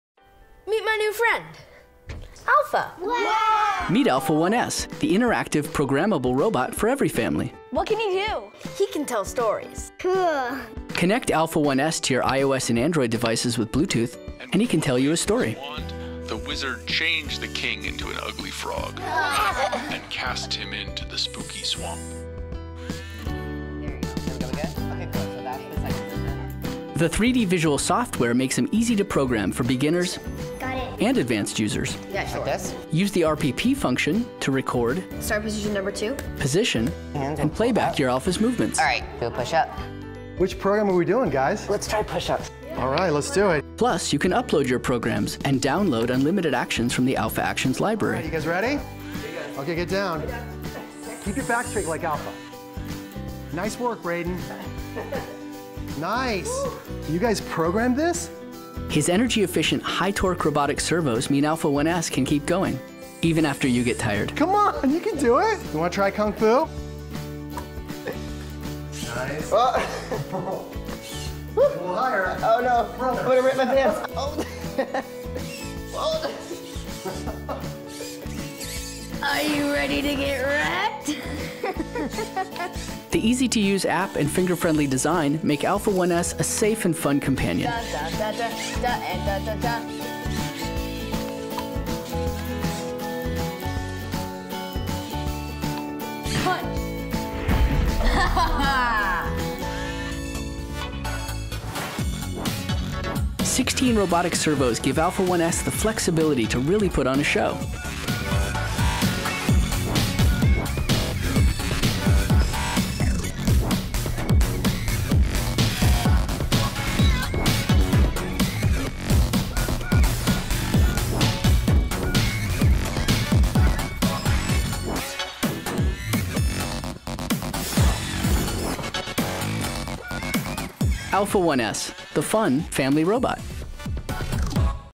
Робот Hoverbot Alpha 1P
Hoverbot ALPHA 1P от компании Ubtech это не просто разумный робот-игрушка, а полноценный член семьи! Данная модель была специально создана с возможностью программирования и системой общения со своим владельцем. Управляйте своим роботом со смартфона или планшета. Просто скачайте бесплатное мобильное приложение от компании Ubtech, подключитесь к вашему роботу посредством bluetooth соединения. Hoverbot Alpha двигается очень реалистично за счёт своих 16 сервоприводов, которые выполняют роль "подвижных суставов". Энергоёмкий аккумулятор позволяет работать без перерыва до 1 часа, а идущее в комплекте зарядное устройство обеспечивает максимально быструю зарядку.